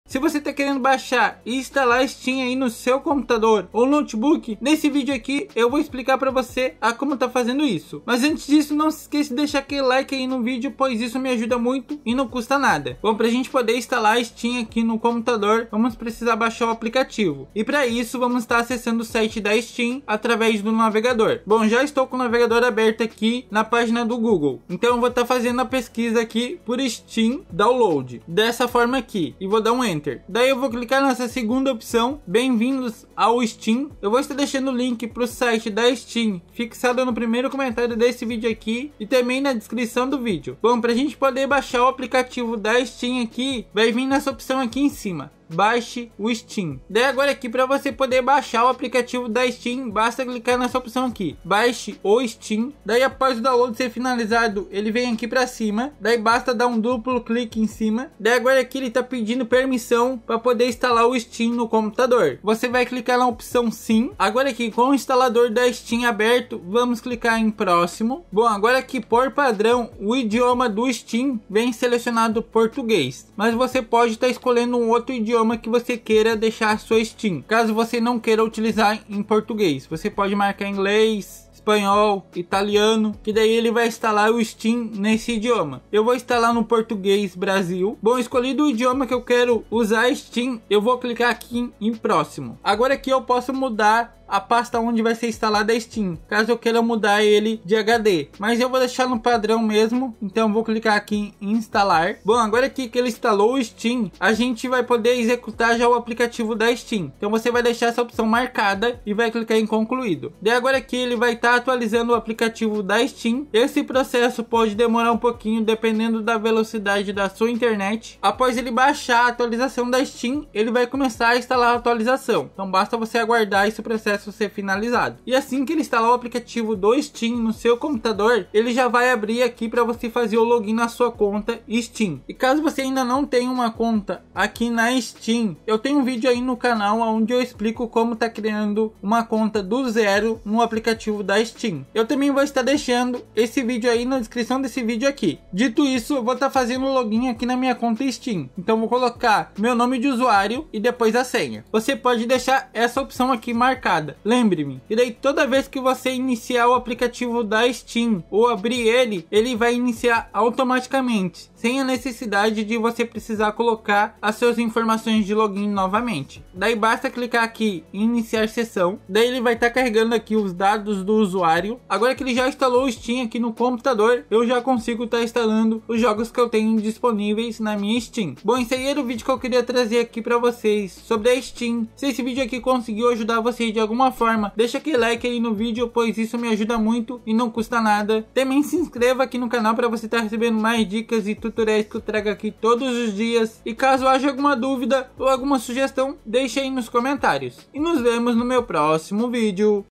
Como INSTALAR STEAM no PC ou NOTEBOOK - Atualizado 2024!
Fala aí pessoal, tudo bem! Sejam bem-vindos a mais um vídeo do canal, onde vou explicar o passo a passo de como baixar e instalar o aplicativo Steam no PC para que você possa baixar, instalar ou até mesmo comprar jogos pelo aplicativo Steam. Confira como é fácil e rápido baixar e instalar o Steam no PC!

MEUS EQUIPAMENTOS:
*Microfone Gamer Hyperx Quadcast S:*